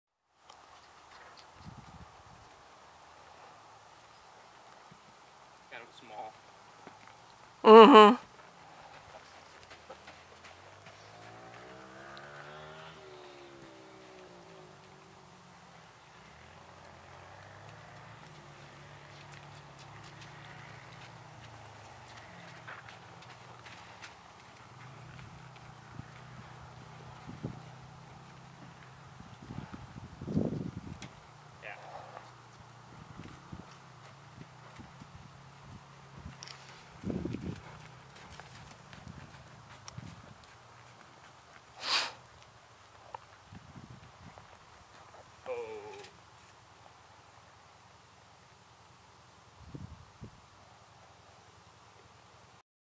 Lilly Trot
Lilly trotting in the round pen.